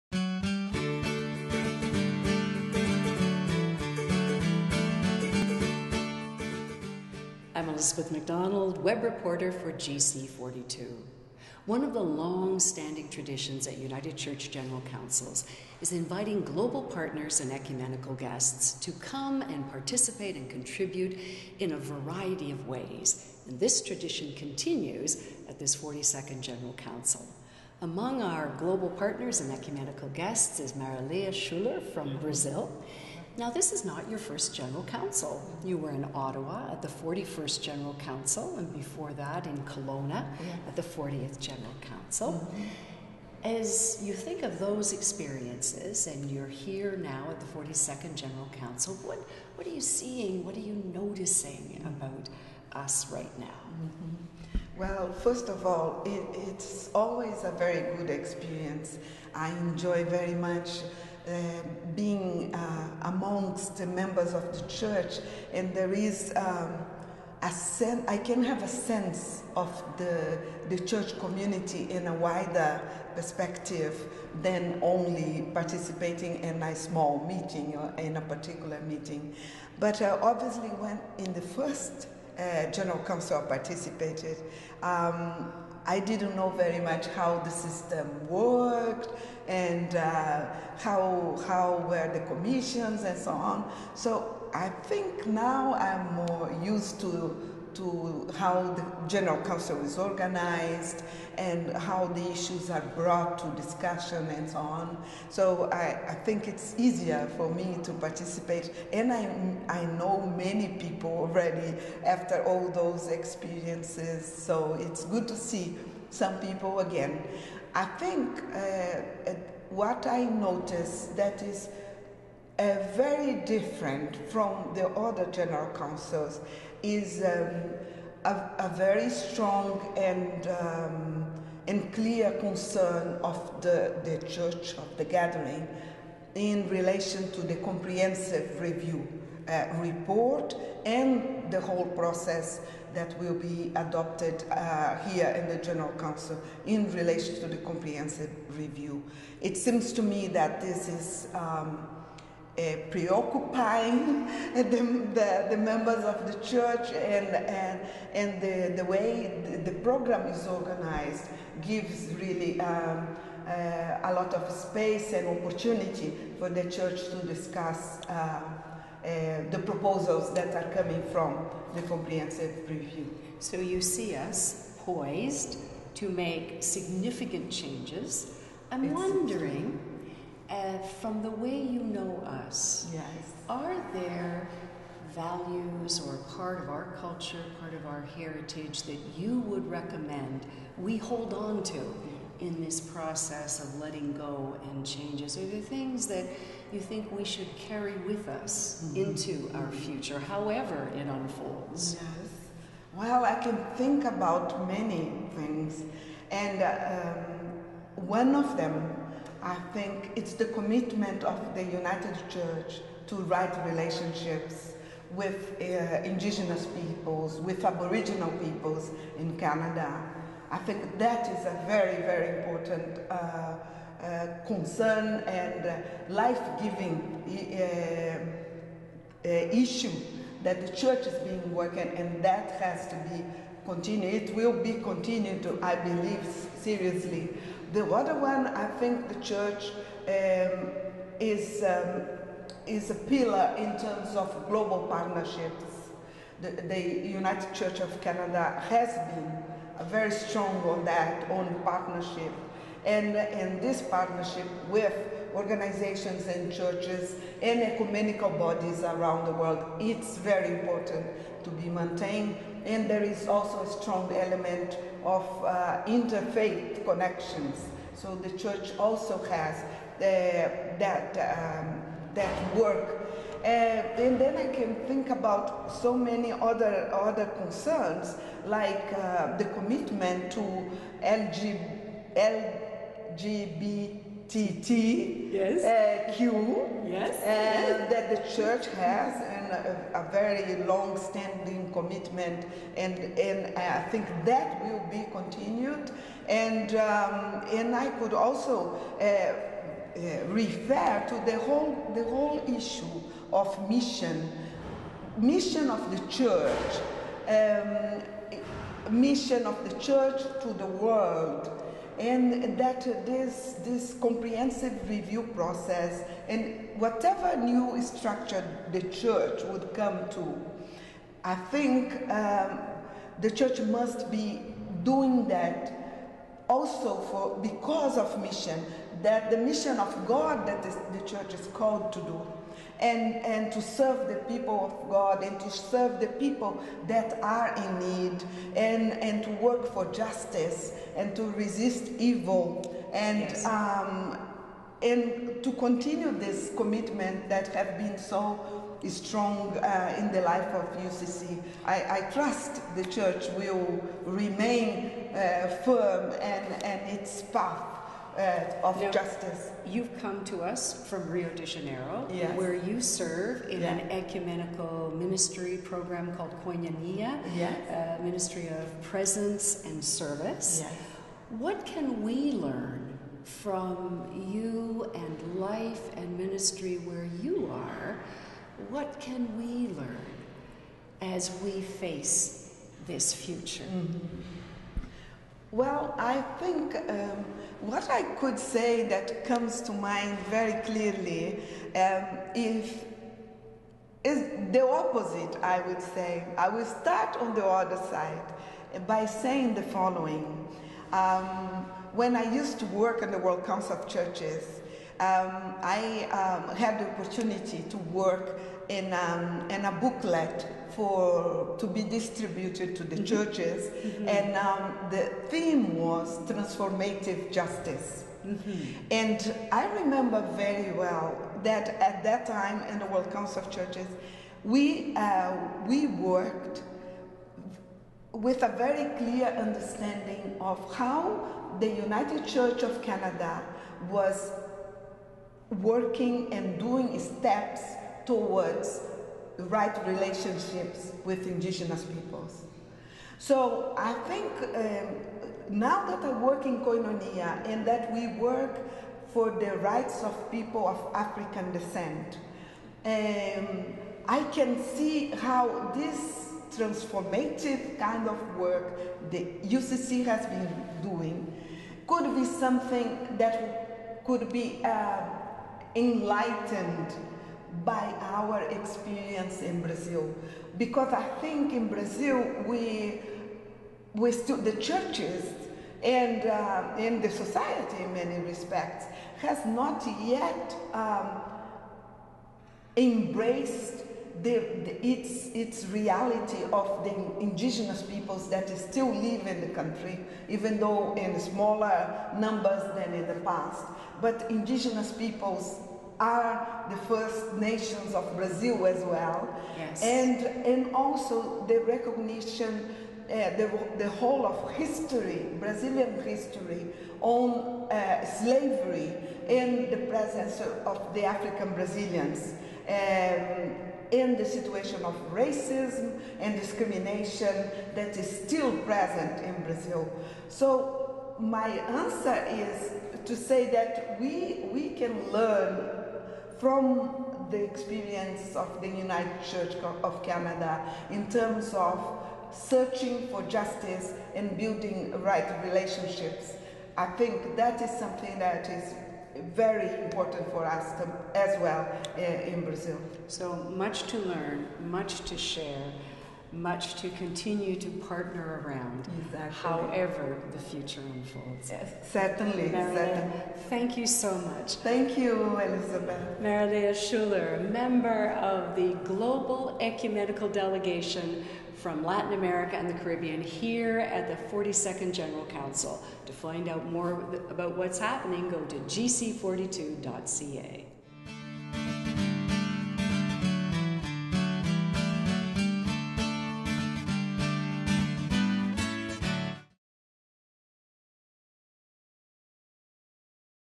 Marilia Schuller, Global Partner
Marilia Schuller,  from the Koinonia community in Brazil, that works with marginalized people of African descent, speaks with Elizabeth Macdonald about the long-standing partnership with the United Church.